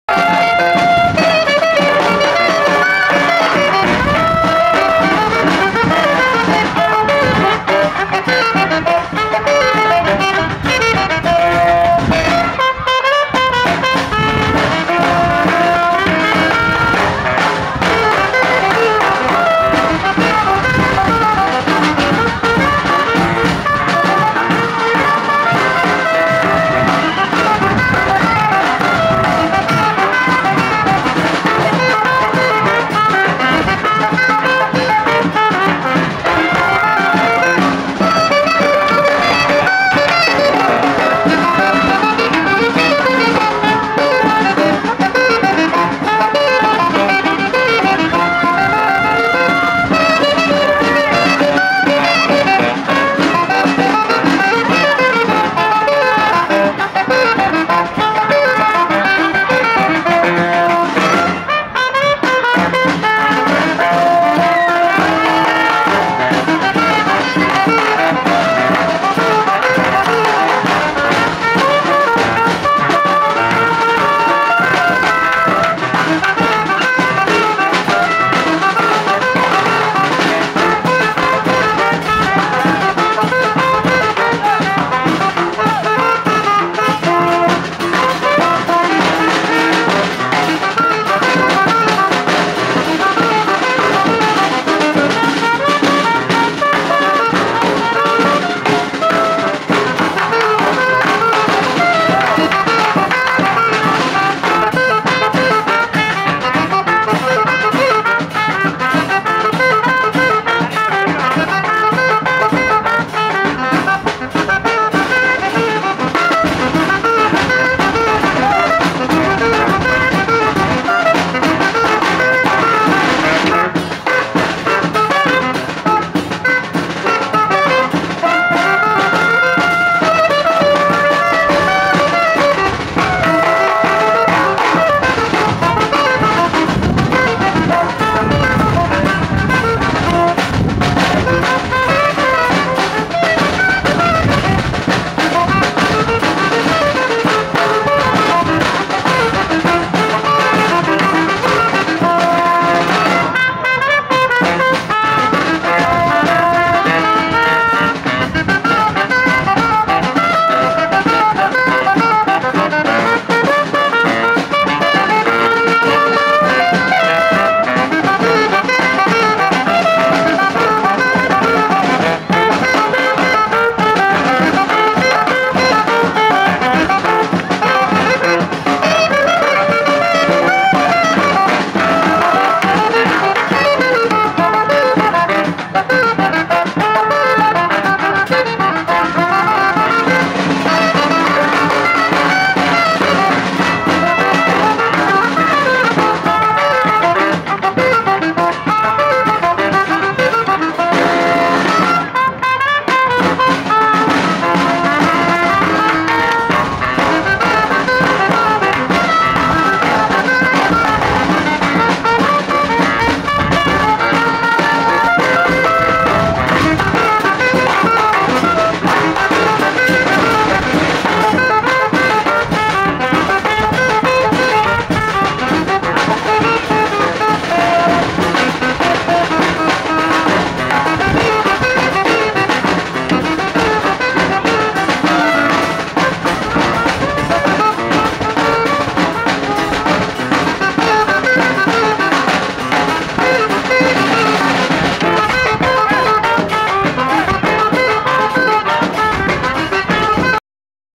3 Kings Parade~NYC~2012~Museo Del Barrio Float~NYCParadelife
Latin jazz band shows how its done at 3 Kings Day Parade in El Barrio, NYC